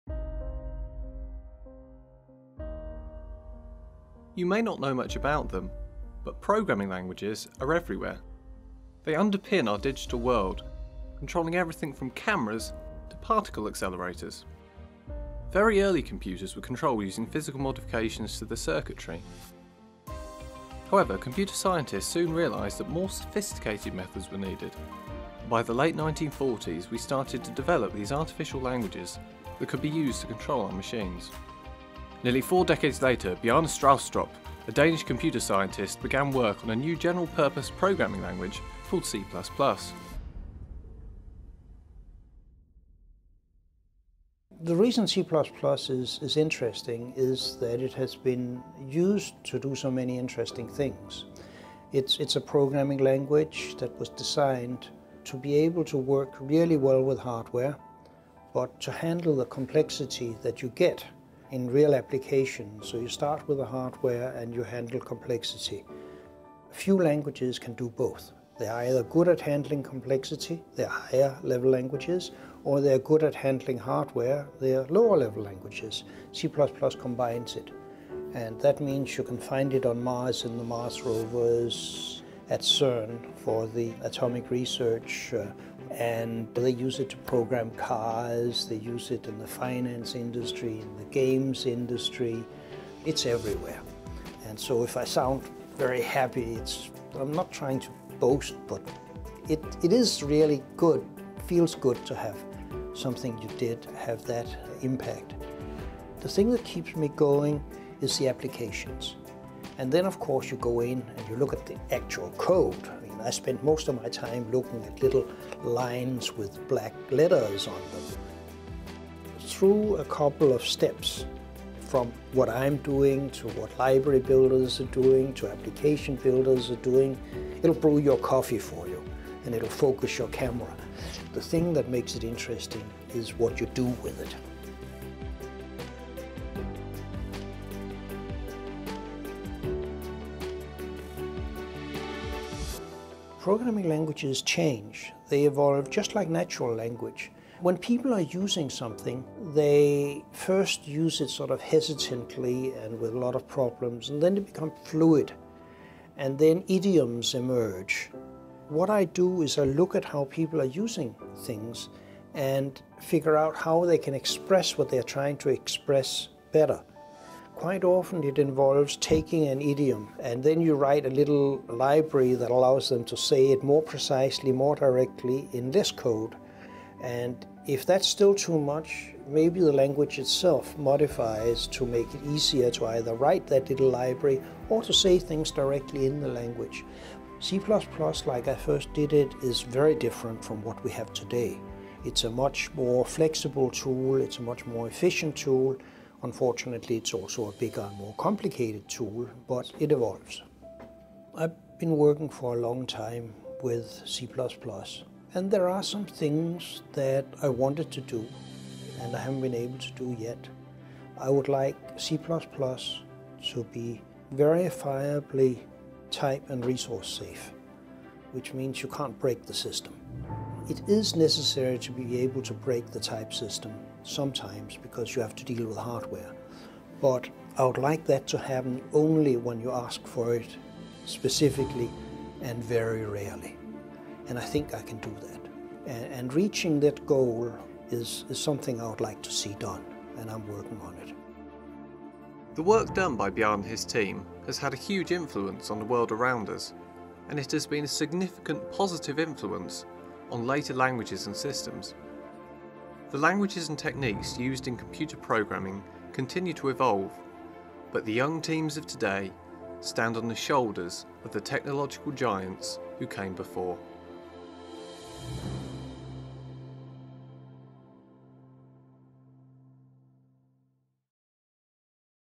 C++: The Evolution of a Programming Language
Programming languages are everywhere. They underpin our digital world, controlling everything from cameras to particle accelerators. Very early computers were controlled using physical modifications to the circuitry, rather than any kind of software interface.  However, computer scientists soon realised that more sophisticated methods were needed, and by the late 1940s we had started to develop these formal languages that could be used to control our machines.

Nearly four decades later, Bjarne Stroustrup, a Danish computer scientist, began working on a new general purpose programming language called C++. Stroustrup found many existing languages either had features that were very helpful for large software development, but remained too slow for practical use, or were fast but too low-level to be suitable for large software development.  He resolved to find a solution.

Several years later, in 1985, the first edition of “The C++ Programming Language” was released.  More than three decades later, it remains one of the most popular and powerful programming languages in the world.